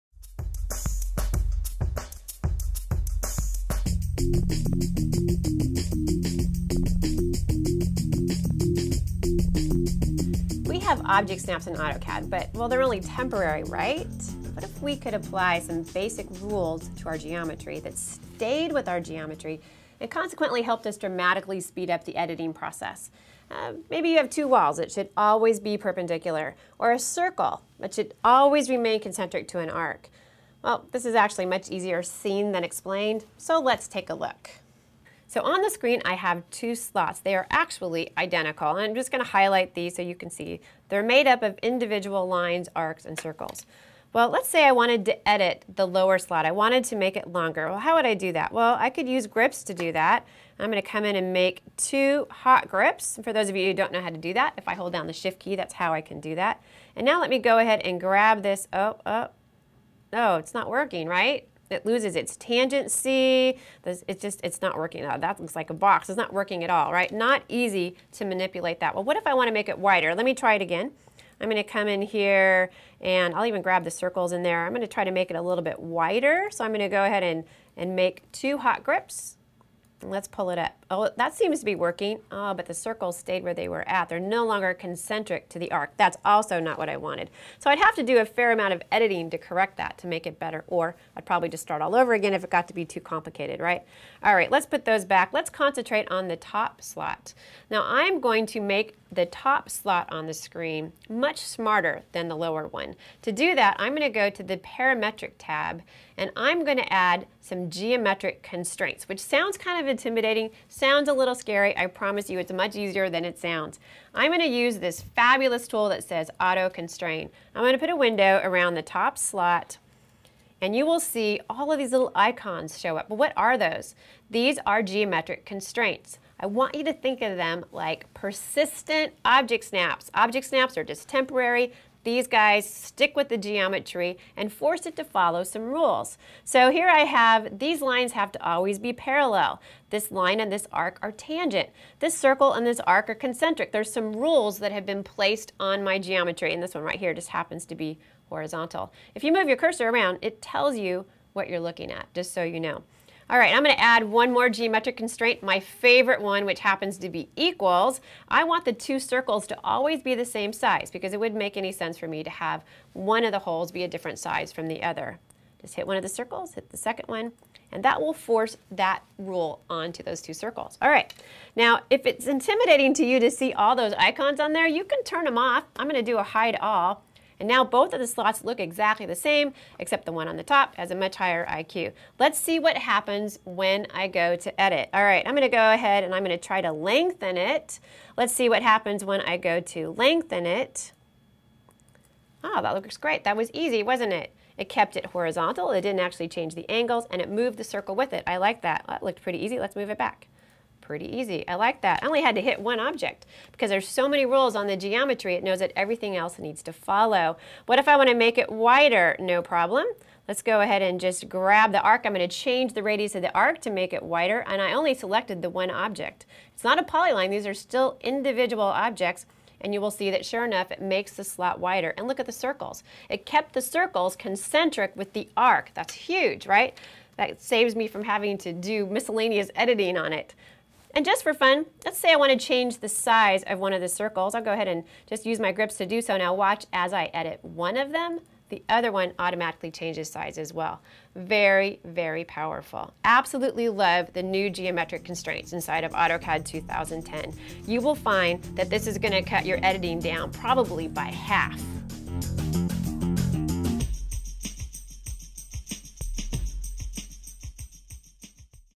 Lynn Allen: AutoCAD 2010 Geometric Constraints
AutoCAD 2010 makes it easy to apply basic rules to the geometry that stay with your drawings, dramatically speeding up the editing process. Lynn Allen shows you how geometric constraints work.